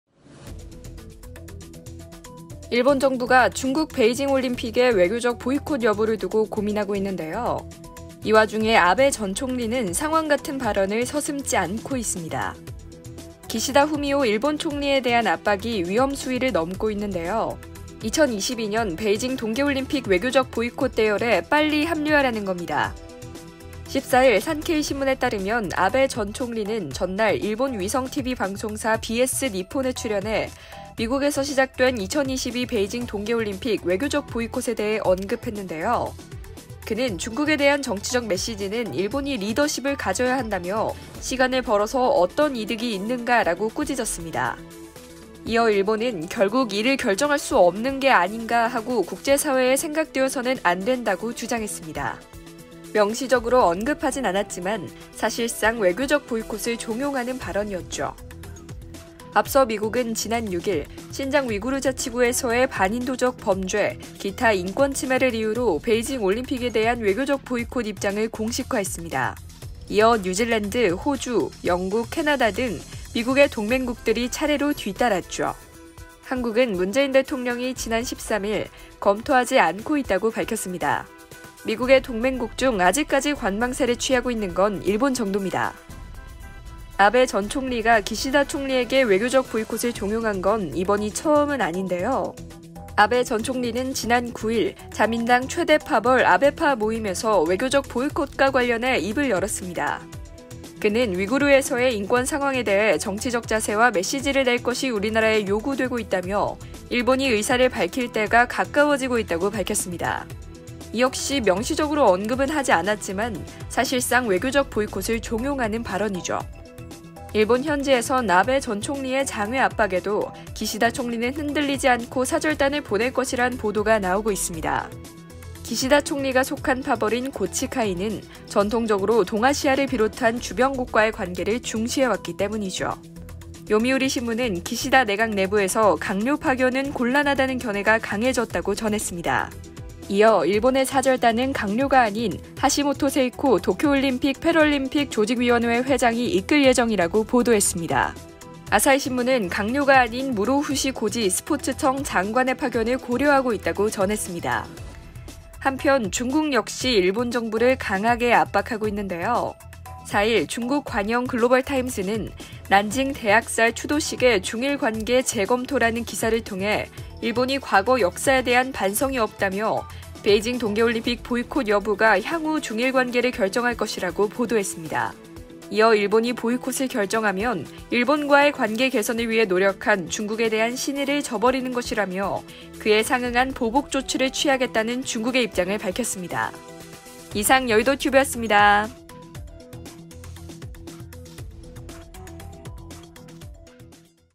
[여의도튜브] “어? 아베가 상왕노릇 하네” 日 ‘中사절단’ 놓고 요지경 /머니투데이방송
일본 정부가 중국 베이징올림픽에 외교적 보이콧 여부를 두고 고민하고 있는데요.
이 와중에 아베 전 총리는 ‘상왕’같은 발언을 서슴지 않고 있습니다. 기시다 후미오 일본 총리에 대한 압박이 위험 수위를 넘고 있는데요,
2022년 베이징 동계올림픽 '외교적 보이콧' 대열에 빨리 합류하라는 겁니다.

아나운서: 김서영
영상콘텐츠: 길인호
기사콘텐츠: 김서영
기획: 강인형
총괄: 정미경
제작: MTN 머니투데이방송

- MTN 투데이